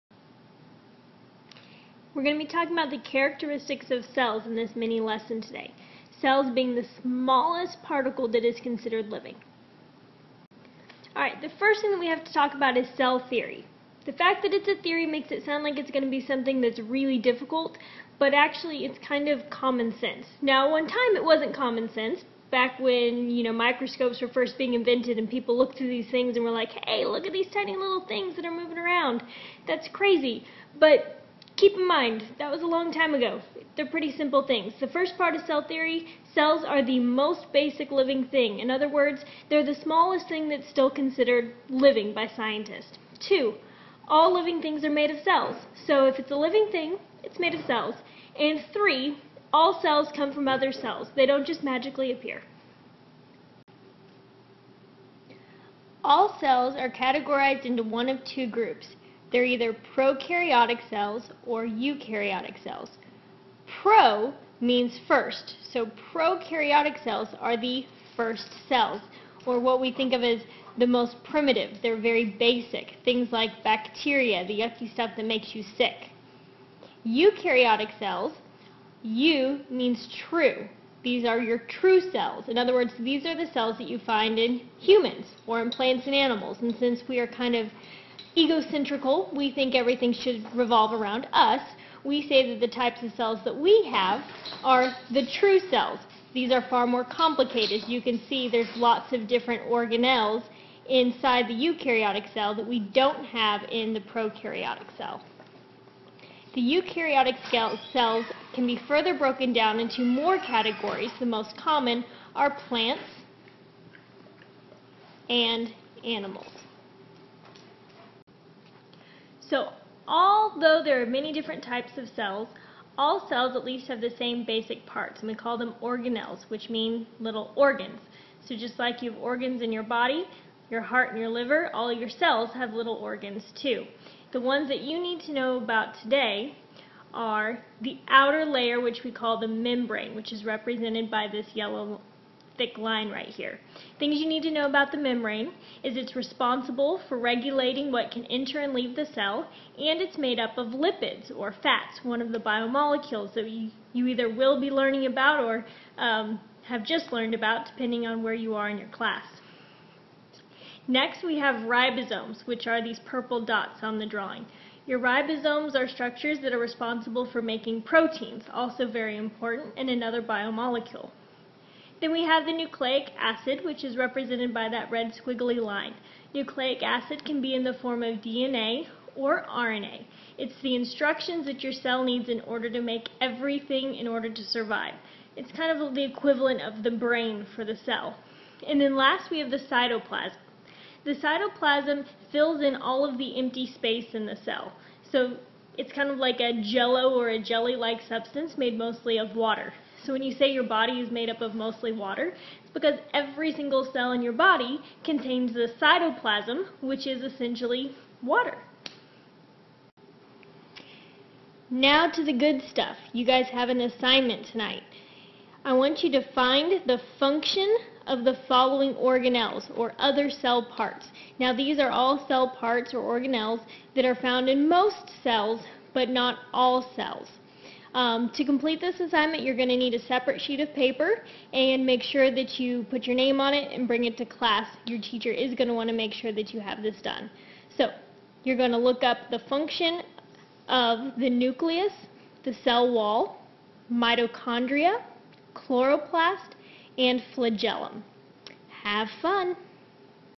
cell introduction lesson
An overview of cells, and cell theory.